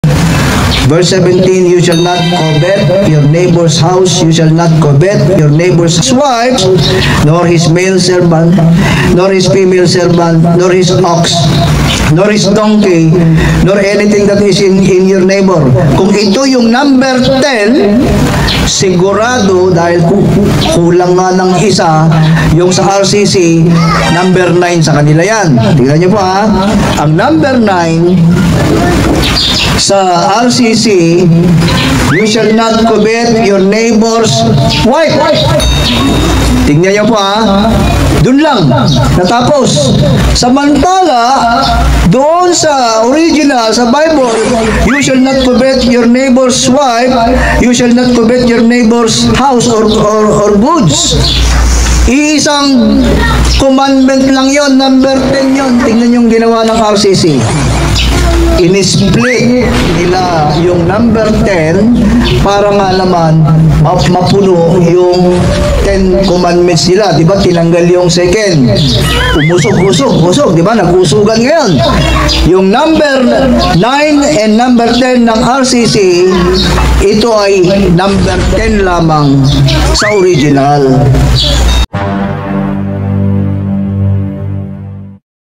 Video Clip No.19/37: ANG NO.10 SA BIBLE AY NAGING NO.9 & 10 SA RCC VERSION PARA MAKUMPLETO ANG LIST!
SOURCE VIDEO: ANG KAHANGALAN NG PAGSAMBA SA DIUS-DIOSAN [The Folly of Idol Worship]
CONTENTS: Comparison between the Biblical Version and the RCC’s